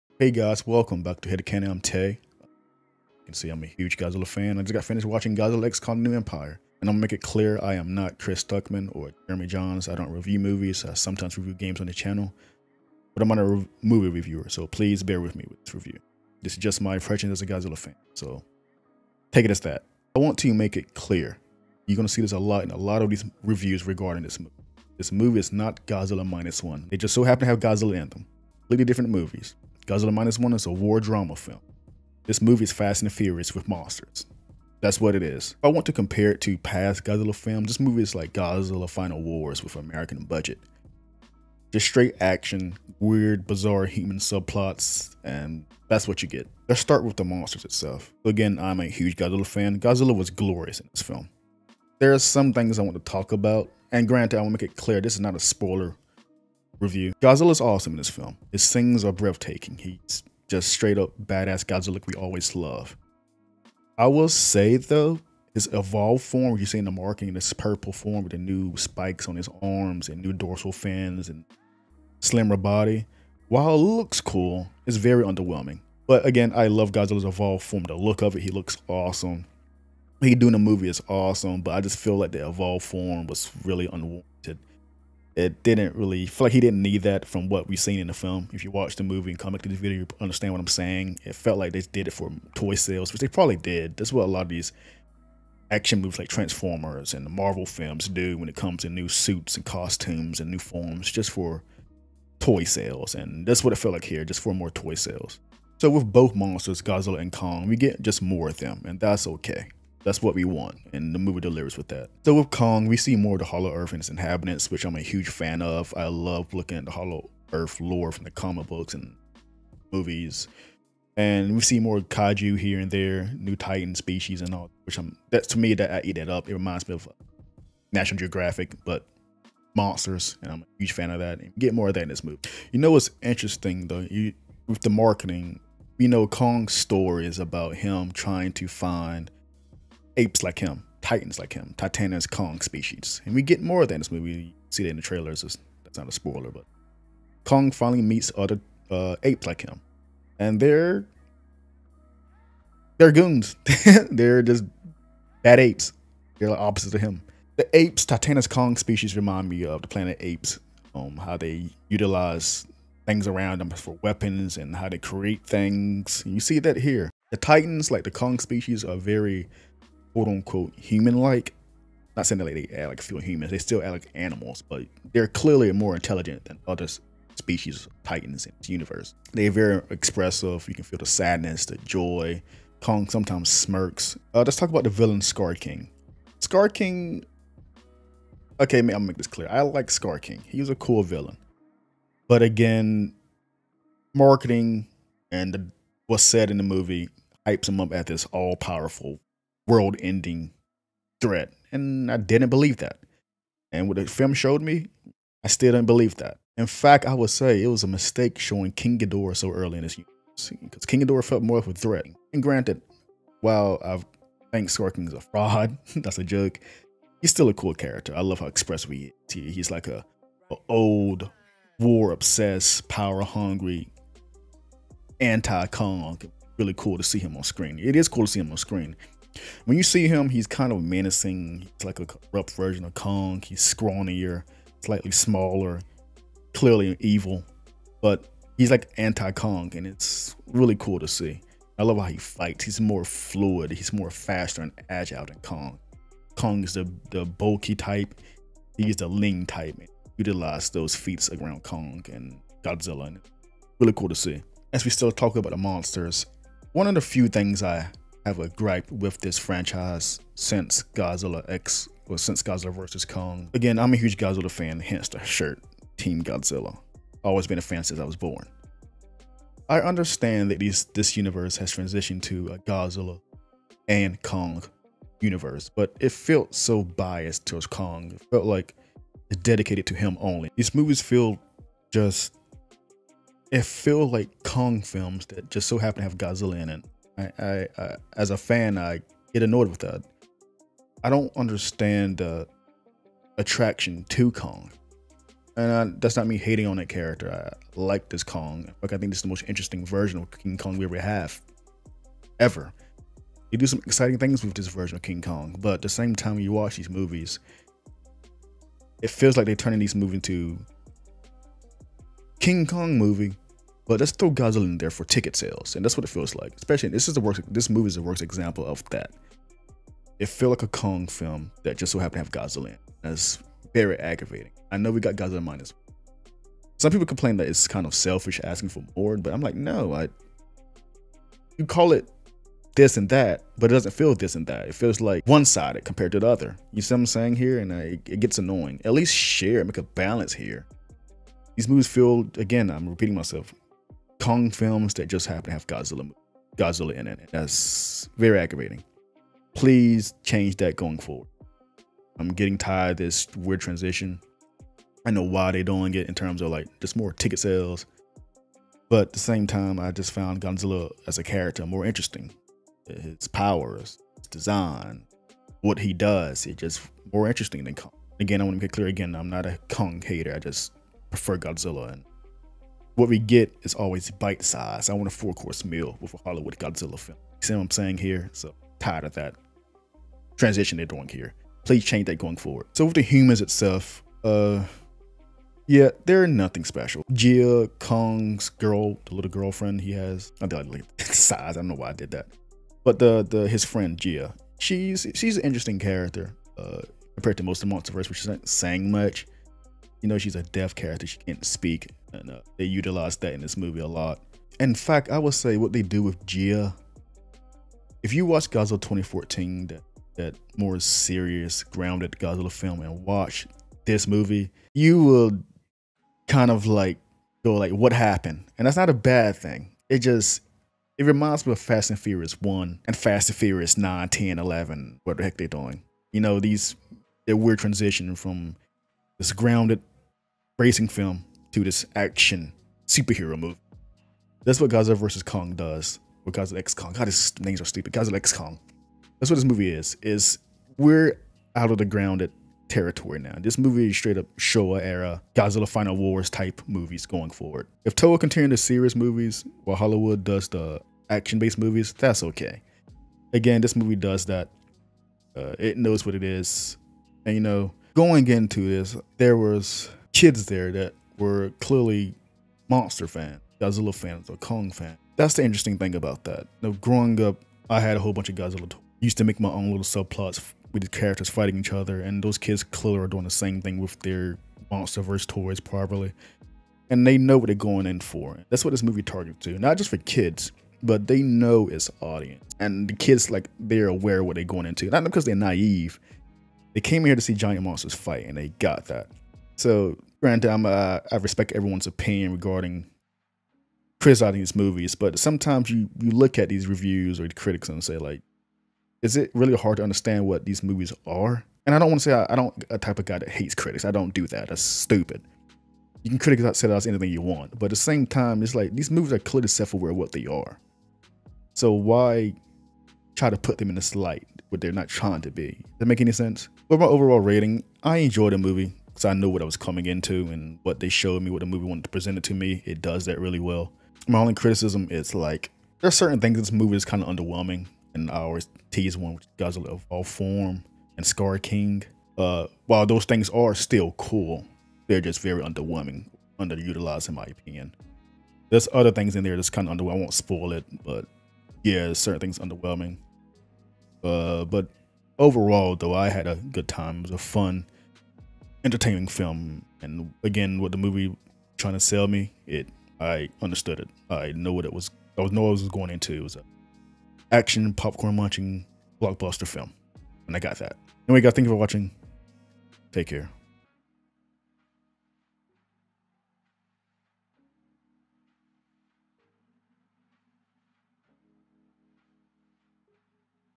Godzilla X Kong: The New Empire Review (NO SPOILERS)
Welcome to the HeadKanon! Tae, Jordan & Bryan discuss various topics revolving pop culture across the internet.